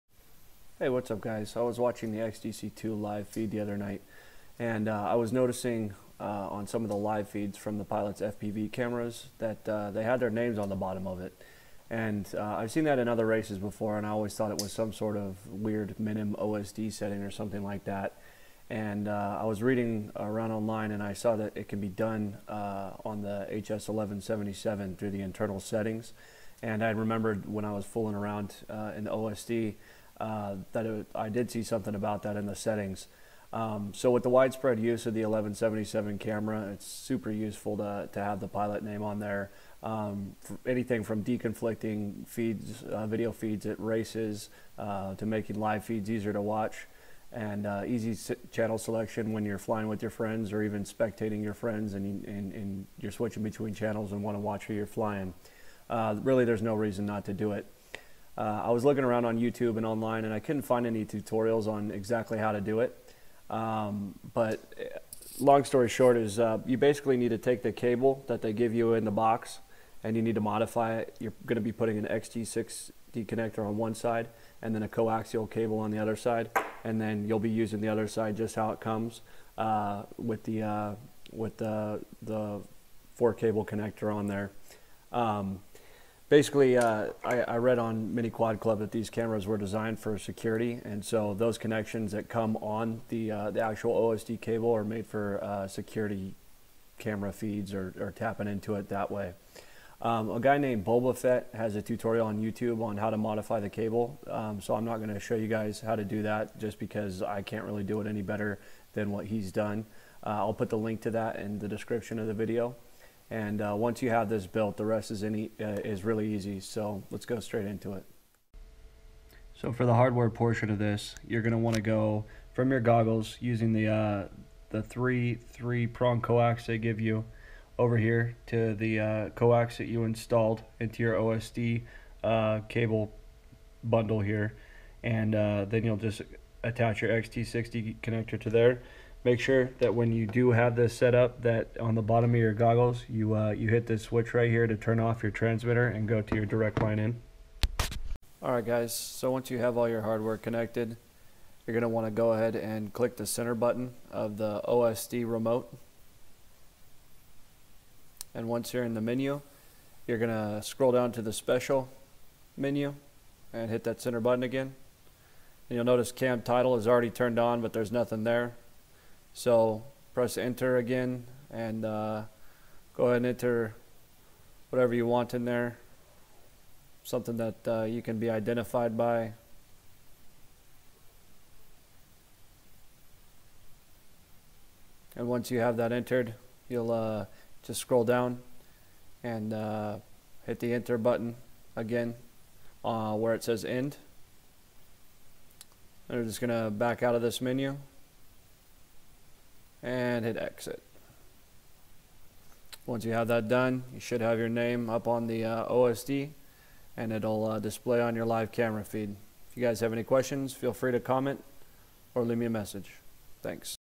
How to put your name on HS1177 camera OSD
Couldn't find a tutorial on this, so I made one!  This is a really easy and useful thing to have on your live FPV feed.